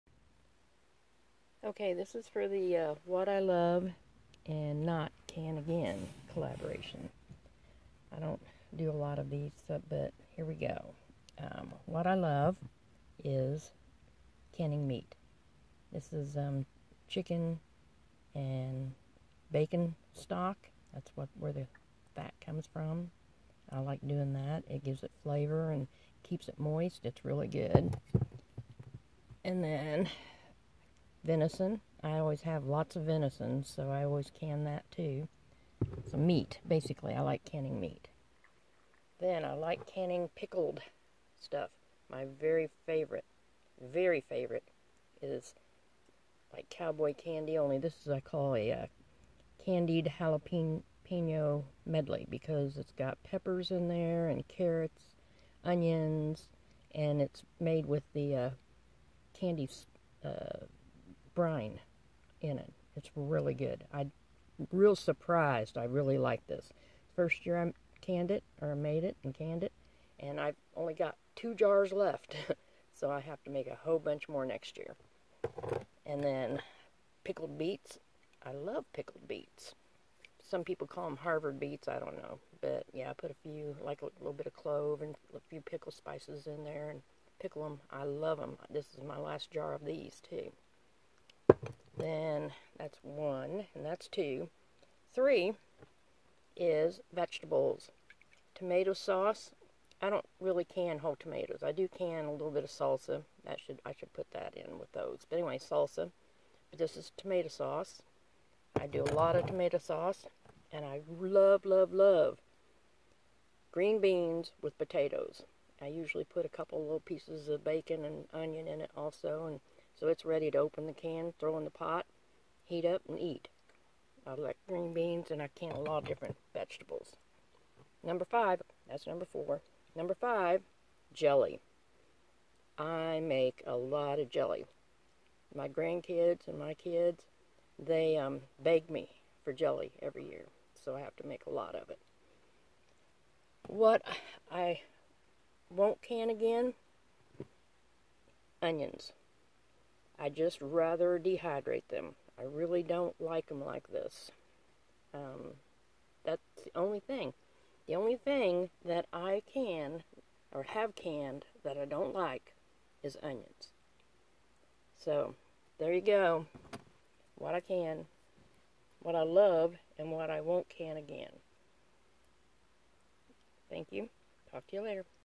What I love & not can again! Collaboration 2022
I decided to join in on this collaboration, it seemed a good way to get recommendations of what works and see what are other folks favorites. I heard about this from Stringfield Ridge Farm. Check out some of my other videos about homestead life.